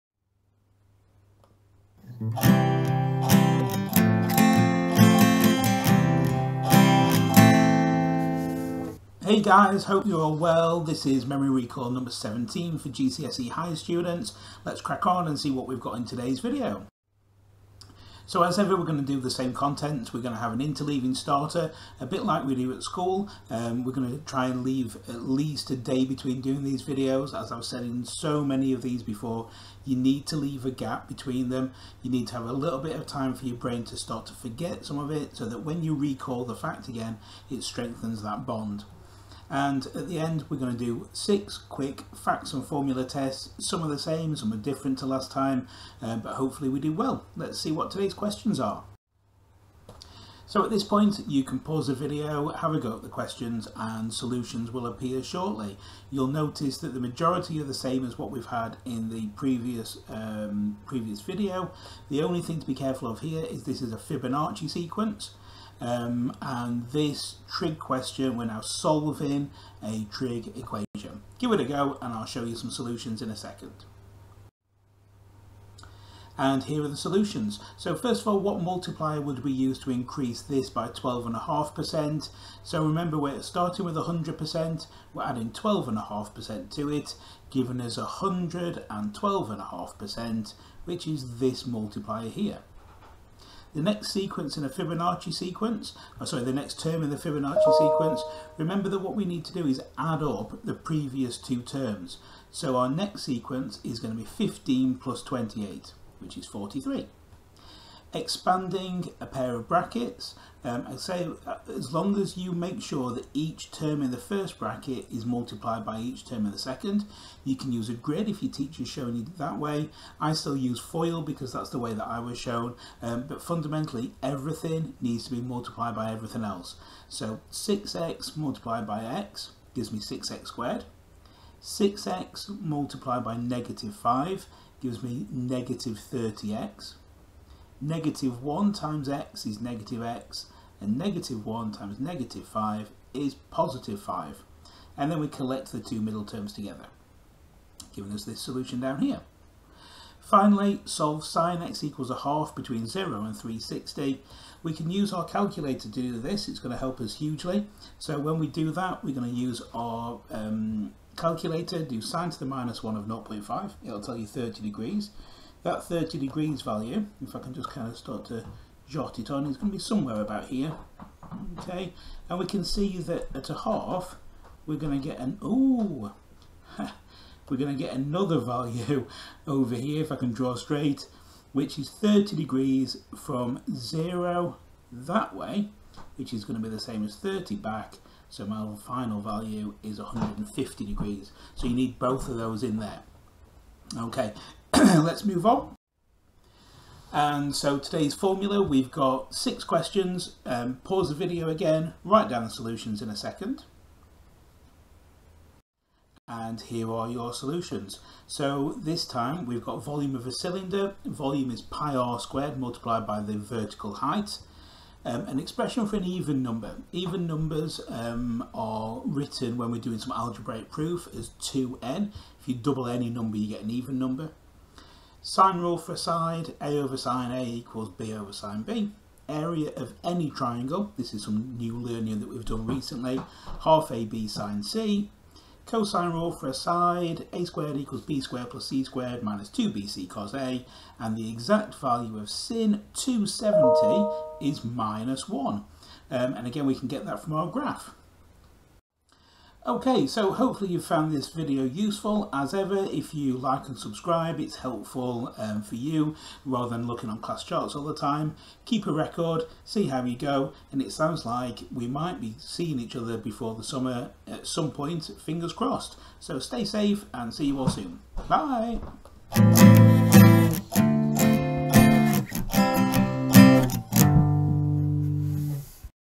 Memory recall 17 - Higher GCSE
More memory recall questions to help you continue to improve your maths skills...